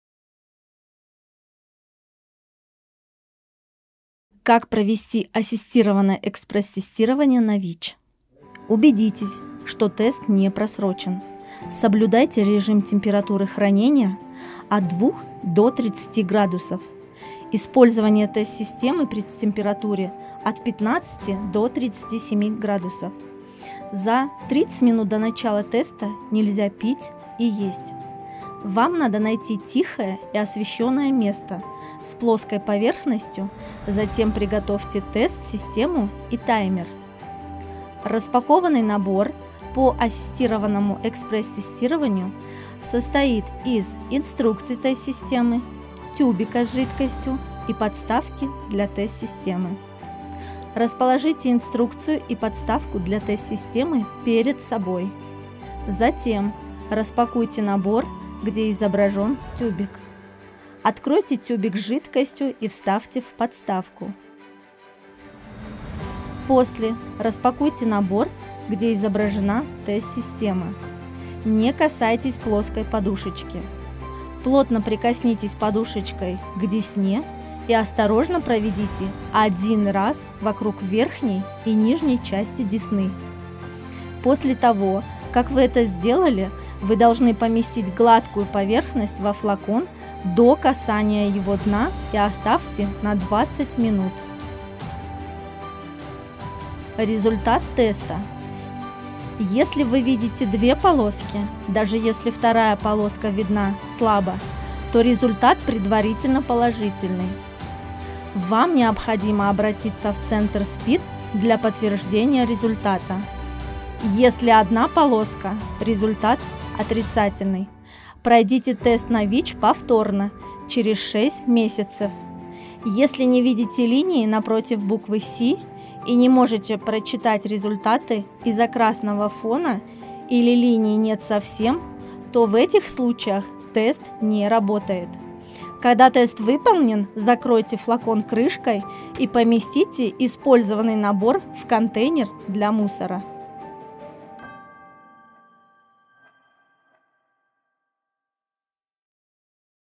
Общественный фонд «Подруга» - Инструкция по ассистированному экспресс тестированию на ВИЧ
Видео было подготовлено Общественным фондом «Подруга» в рамках проекта программе малых грантов проекта "Услуги в сфере ВИЧ под руководством и на базе сообществ - ключ к прекращению эпидемии ВИЧ в Европе и Центральной Азии"